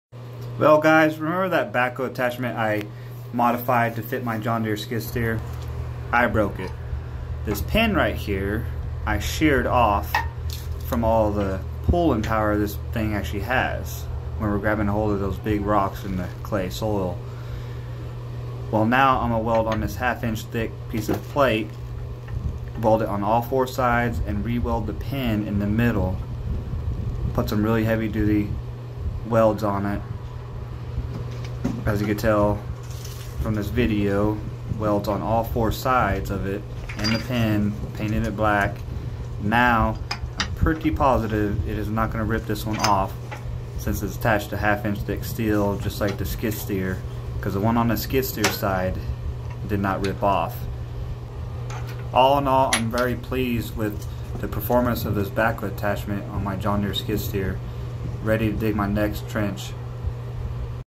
Broke backhoe attachment
Well its a good thing I'm pretty much good at everything. 😆
My backhoe attachment broke, the pins I welded on the side ripped off of each side, because of the pressure when we hooked big rocks.
Well I fixed it even better,  there is no way they're coming off this time.  Other than that very impressed with the performance of the attachment.

Digging a 70' trench tomorrow so let's see.